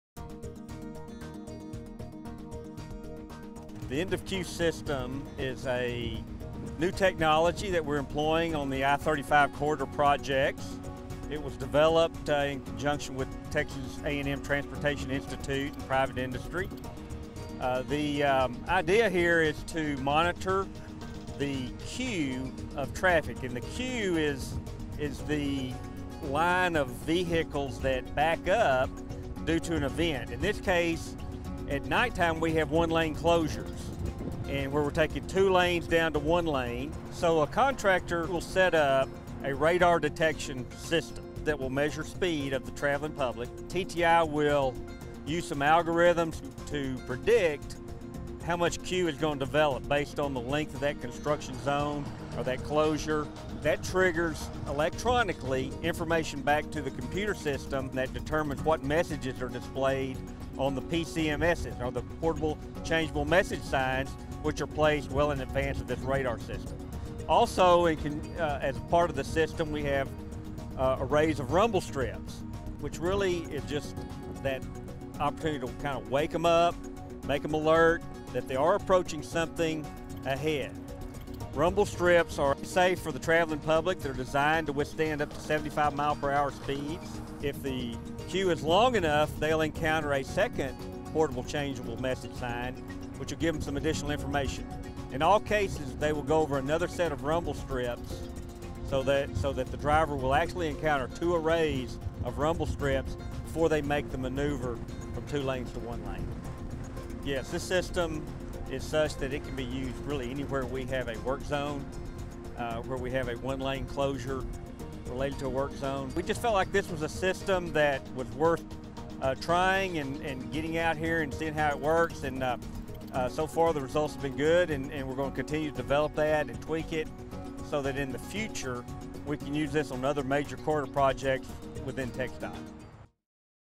End of Queue
TxDOT's "End of Queue" system on I-35 through the Waco District. Sensors detect how long it takes for vehicles to get through a certain stretch of the highway. That data is then automatically displayed on a dynamic message sign for drivers heading toward that same stretch of road to let them know how long it will take.